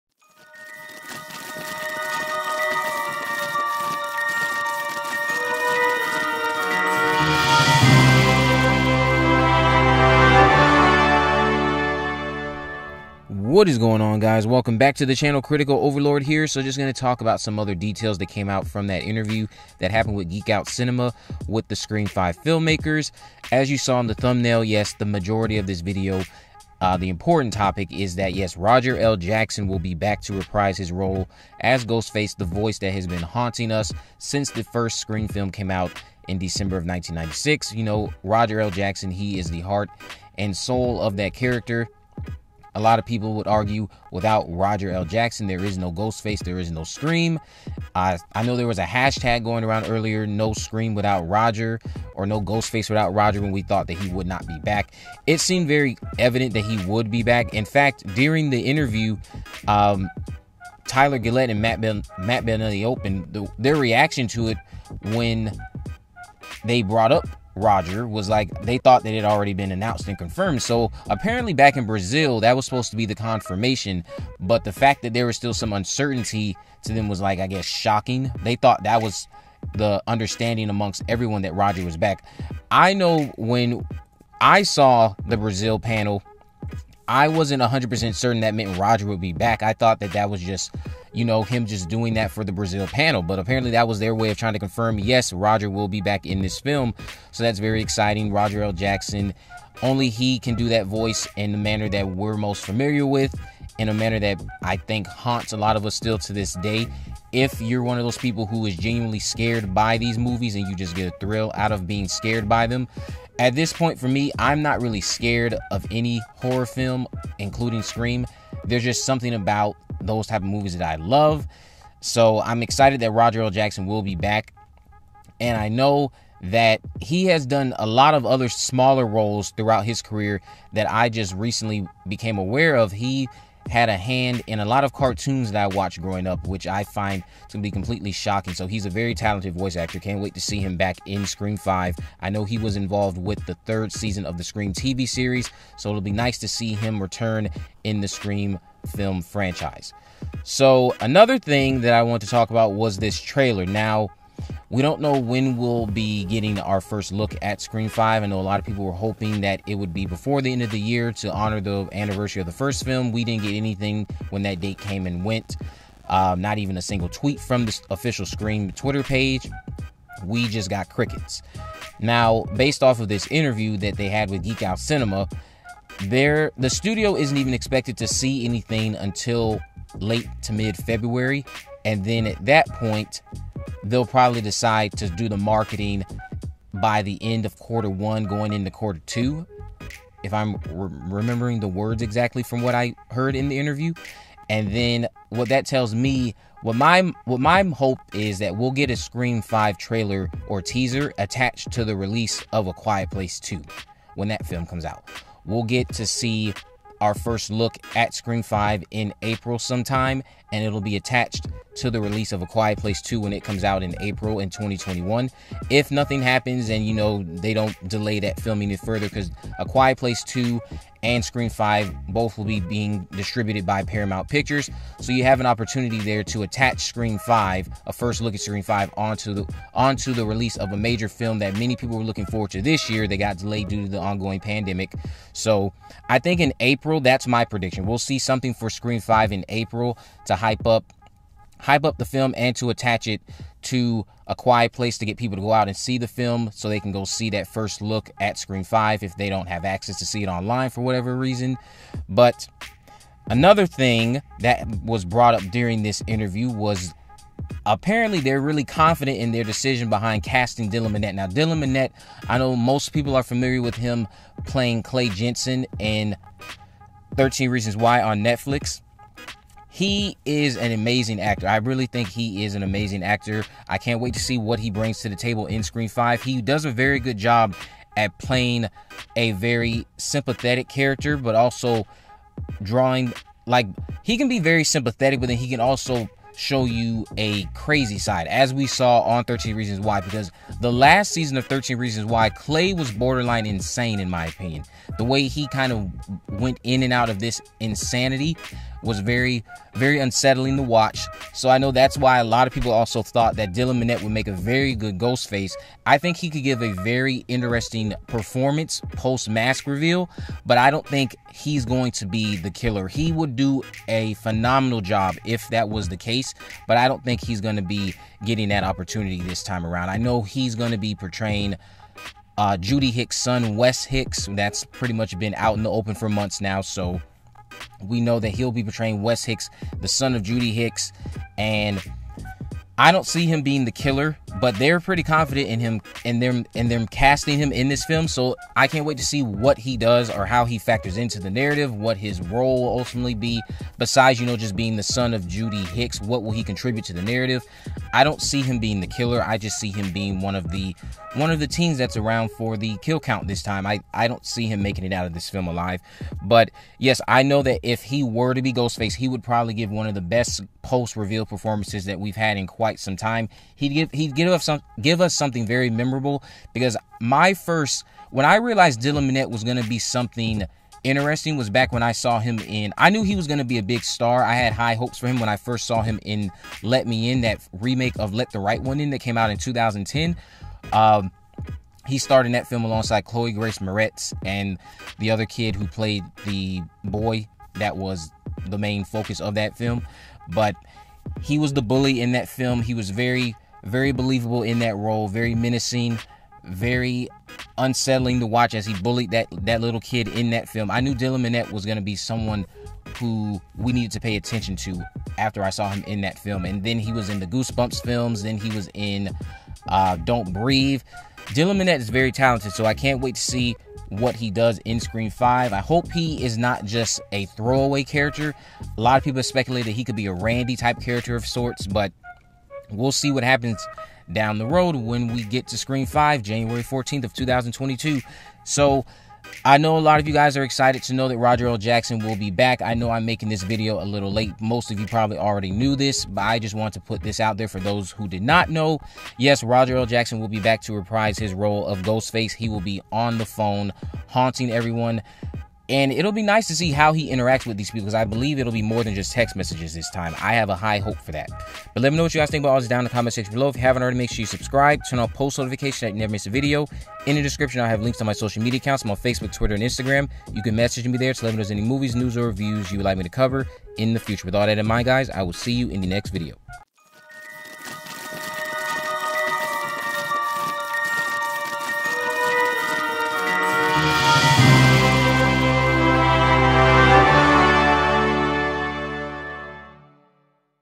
Scream 5 | First Trailer SOON + Roger Jackson Confirmed
Scream 5 filmmakers talked about other topics during the interview with geek out cinema and it seems like a trailer could be arriving by April (My Prediction) and Roger Jackson is confirmed back. Also, Dylan Minnette is getting praised, so I think his performance in Scream 5 will be good. The original voice is back so everyone can relax now.